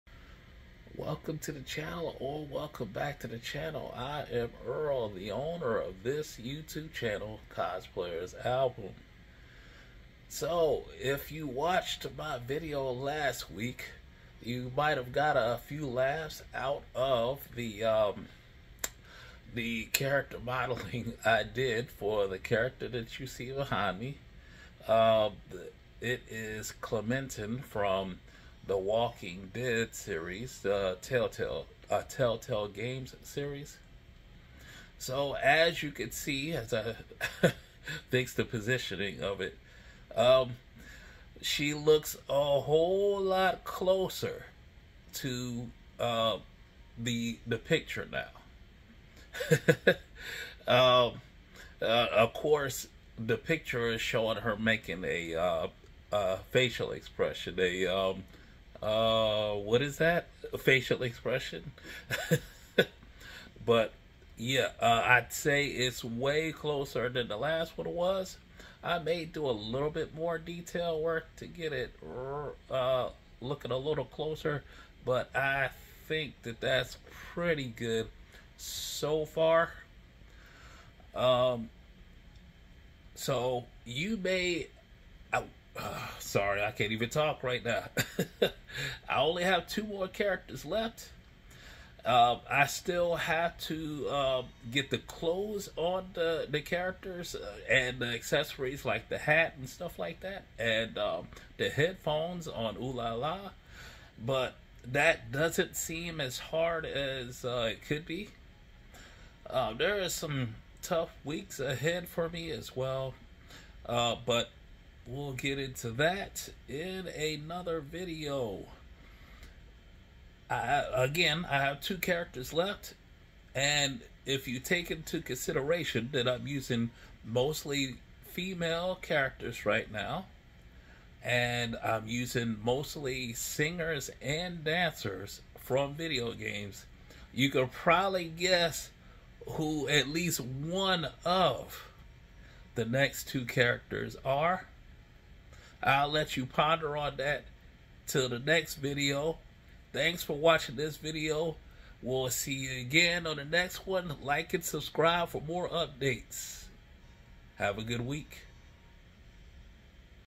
3D Design Video PT24 Reintroducing the 3rd character 🎮
As you can see, the character model has improved dramatically. Good day!